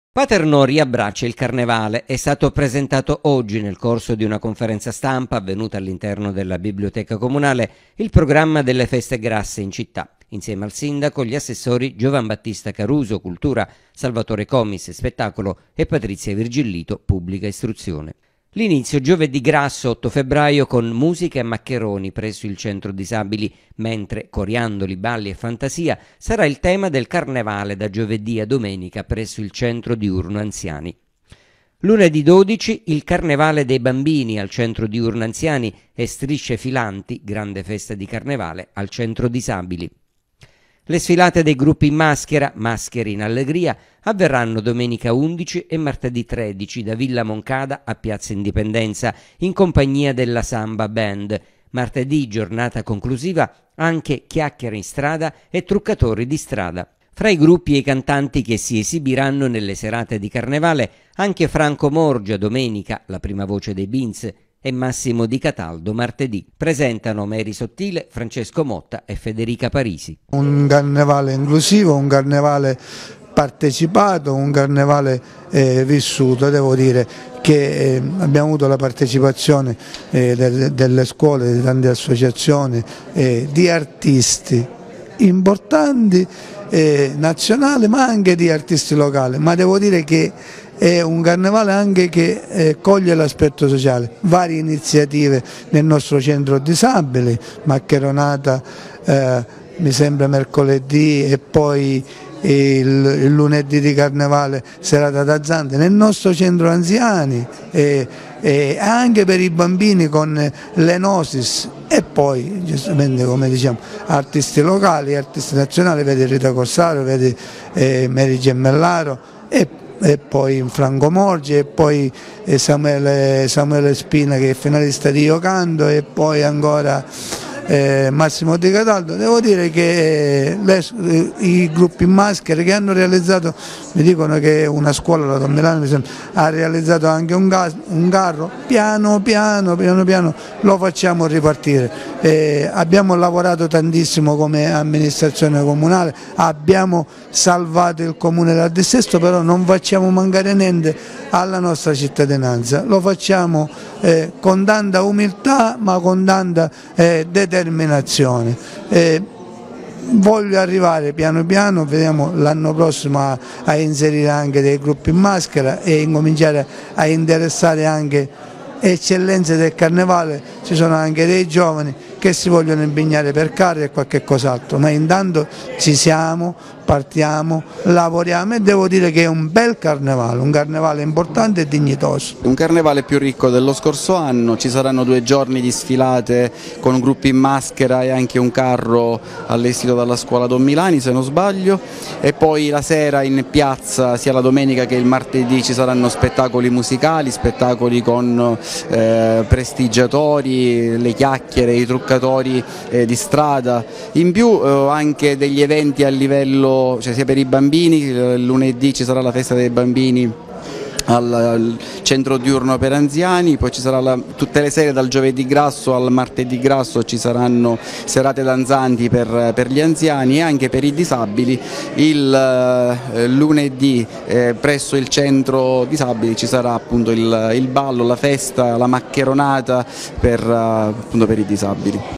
Paternò, presentato il Carnevale
Presentato oggi alla biblioteca comunale, il Carnevale di Paternò 2024. Spettacoli e sfilate accompagneranno i giorni grassi.